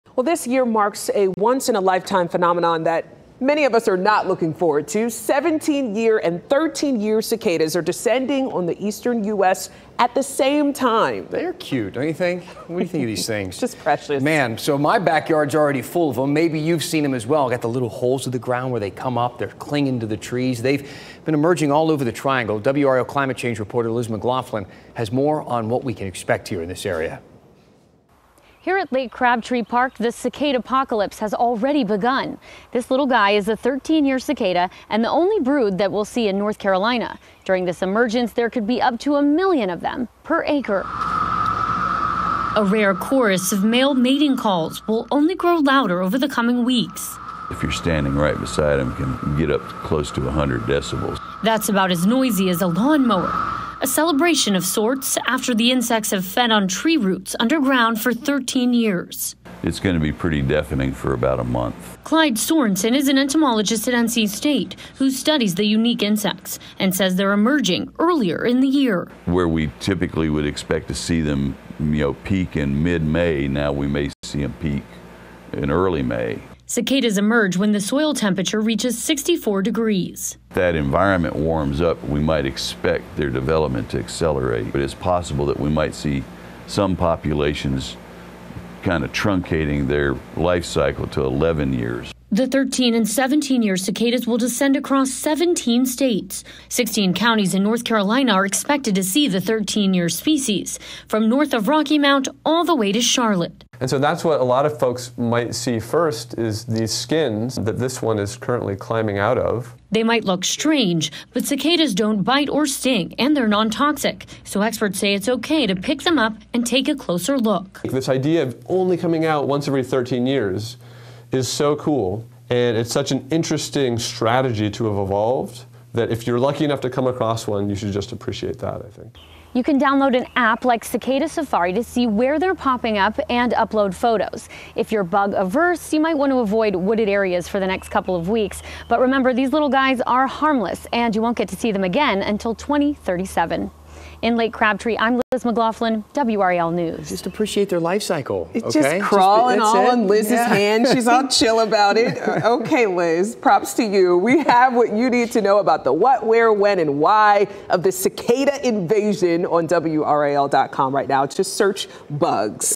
Millions of cicadas emerging in North Carolina
Are cicadas a spectacular wonder of nature or an anxiety-inducing nuisance? It depends on whom you ask.